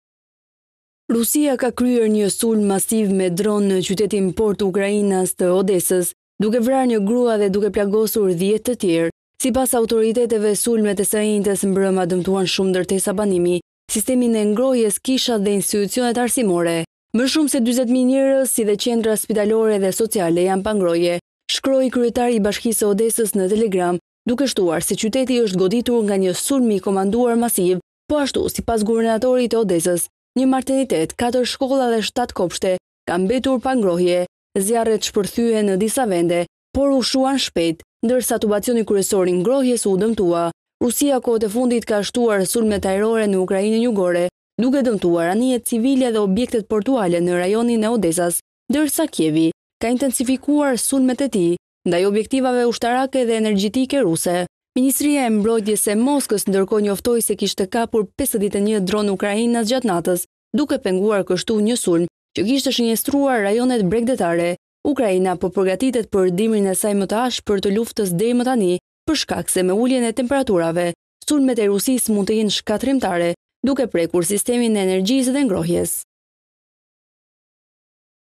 Top News/ Rusia 'i fik dritat' Ukrainës/ Bombardime masive në Odesa, 40 mijë vetë pa energji
Rusia ka kryer një sulm masiv me dronë në qytetin-port ukrainas të Odesës, duke vrarë një grua dhe duke plagosur dhjetë të tjerë. Sipas autoriteteve sulmet e së enjtes mbrëma dëmtuan shumë ndërtesa banimi, sistemin e ngrohjes, kishat dhe institucionet arsimore.
 “Më shumë se 40,000 njerëz si dhe qendra spitaore dhe sociale janë pa ngrohje”, shkroi kryetari i bashkisë së Odesës në Telegram, duke shtuar se qyteti është goditur nga një sulm i kombinuar masiv.
Po ashtu, sipas guvernatorit të Odesës, një maternitet, katër shkolla dhe shtatë kopshte kanë mbetur pa ngrohje. Zjarret shpërthyen në disa vende, por u shuan shpejt, ndërsa tubacioni kryesor i ngrohjes u dëmtua.
Rusia kohët e fundit ka shtuar sulmet ajrore në Ukrainën jugore, duke dëmtuar anijet civile dhe objektet portuale në rajonin e Odesa-s, ndërsa Kievi ka intensifikuar sulmet e tij ndaj objektivave ushtarake dhe energjetike ruse.
Ministria e Mbrojtjes e Moskës ndërkohë njoftoi se kishte kapur 51 dronë ukrainas gjatë natës, duke penguar një sulm që kishte shënjestruar rajonet bregdetare.
Ukraina po përgatitet për dimrin e saj më të ashpër të luftës deri më tani, për shkak se me uljen e temperaturave, sulmet e Rusisë mund të jetë shkatrrimtare duke prekur sistemin e energjisë dhe ngrohjes.

Pamje:AFP